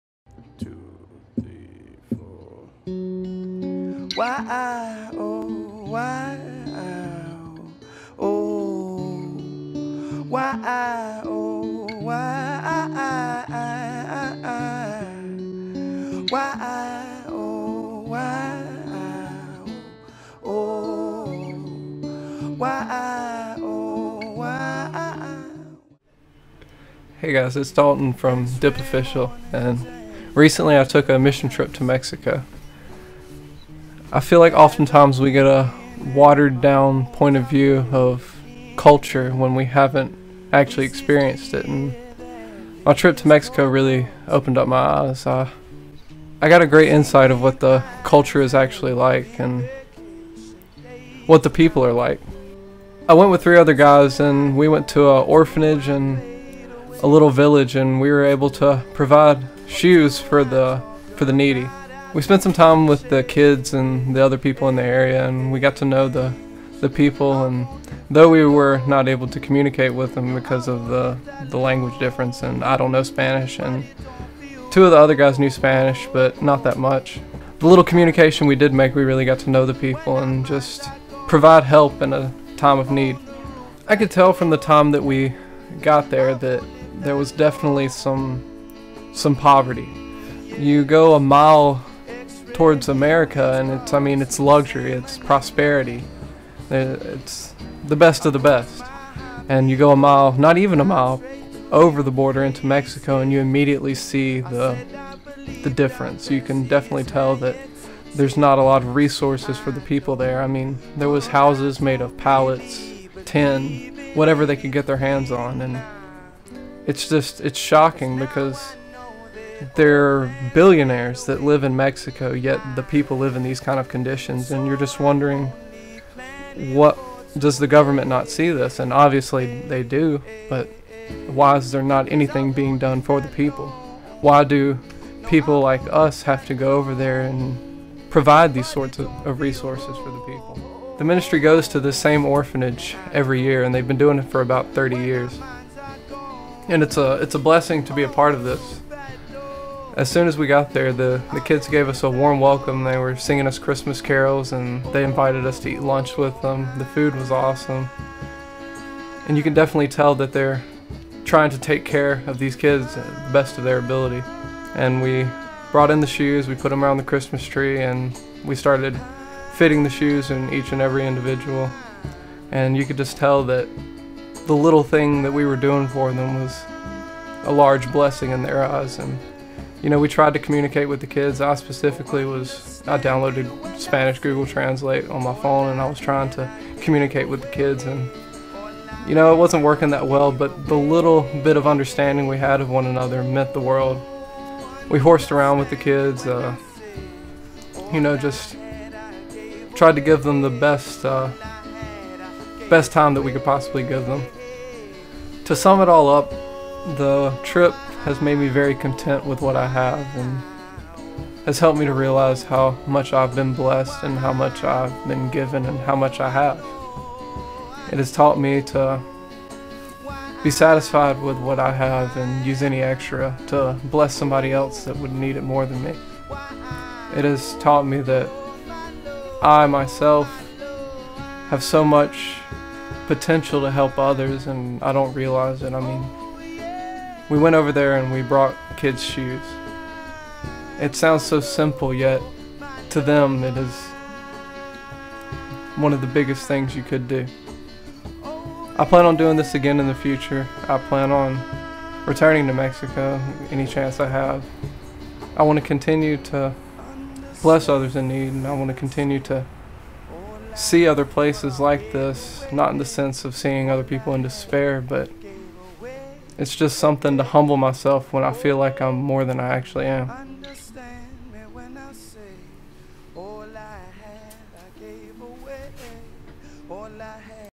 mission in mexico
Dalton went on a mission trip and shares his experiences and how it effected him.